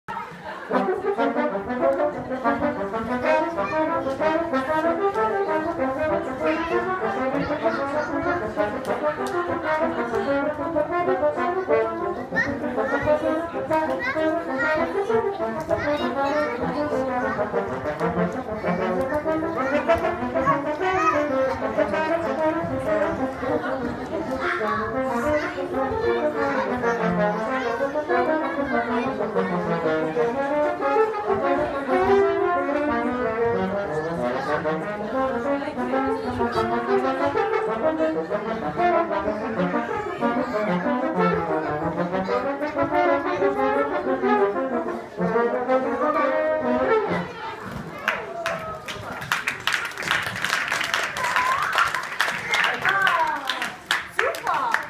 Bippery Bop Horns!
Klaus Wallendorf and Sarah Willis played a Bippery and the kids bopped!